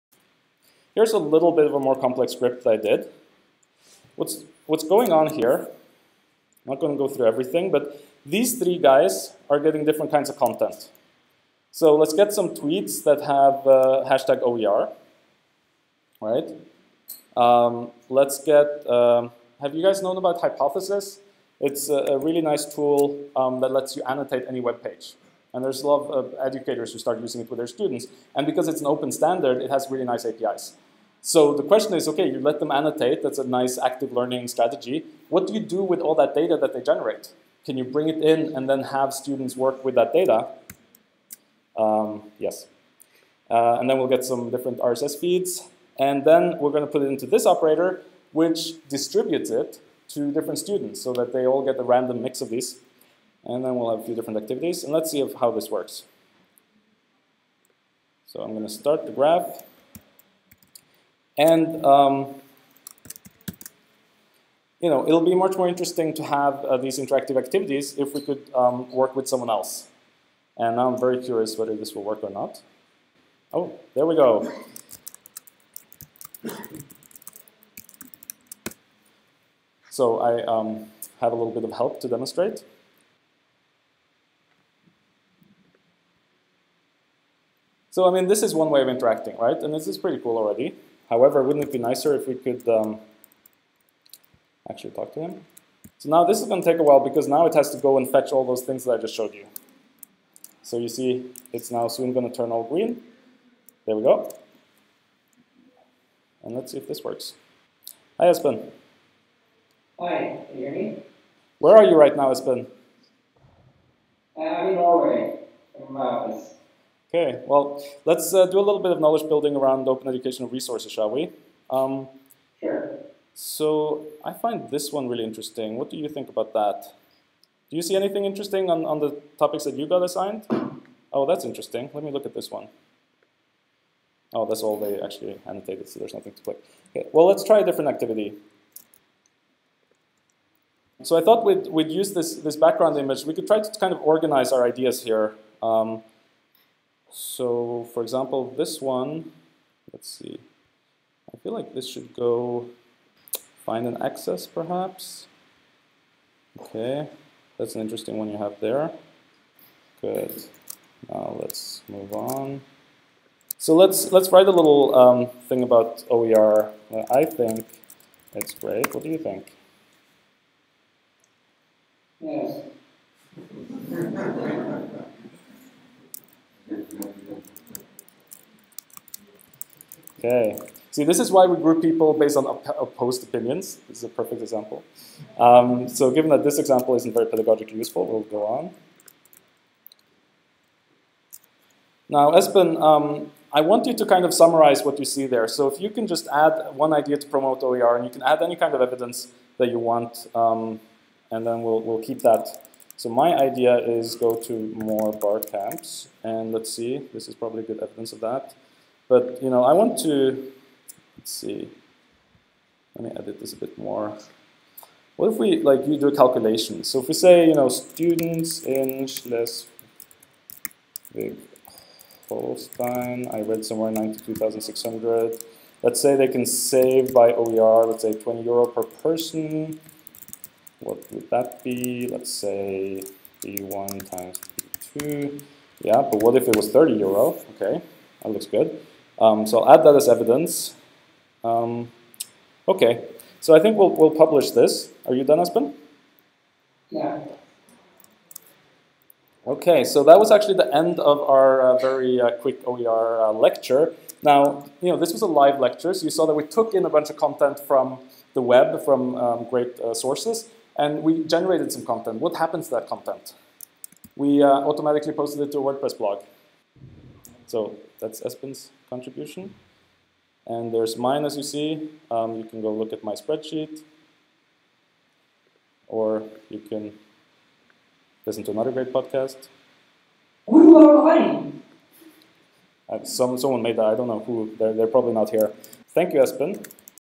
Live demo from FROG Lübeck
Only the live-demo part of talk "FROG: Embeddable tools for rich collaborative learning" at Virtuelle Fachhochschule Symposium 2018 in Lübeck.